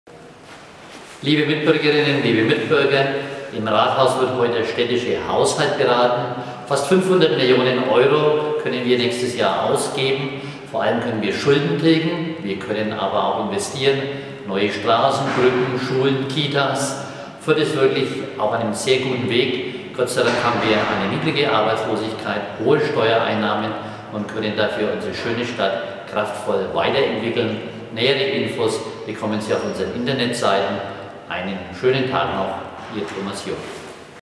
OB zu den Haushaltsberatungen 2017
Heute beraten die Fürther Stadträtinnen und Stadträte im Rathaus über den Etatentwurf für das kommende Jahr, den die Kämmerei vorbereitet und vorgelegt hat. Über die Rahmenbedingungen zeigten sich Oberbürgermeister Thomas Jung und Finanzreferentin Stephanie Ammon zufrieden. Denn nach 2014 (2,4 Millionen Euro), 2015 und 2016 (jeweils mehr drei Millionen Euro) gelingt es aller Voraussicht nach auch im 2017 Schulden abzubauen. Diesmal liegt der Betrag sogar bei sieben Millionen Euro.
Ein kurzes Statement des Fürther Rathauschefs zum Haushalt 2017.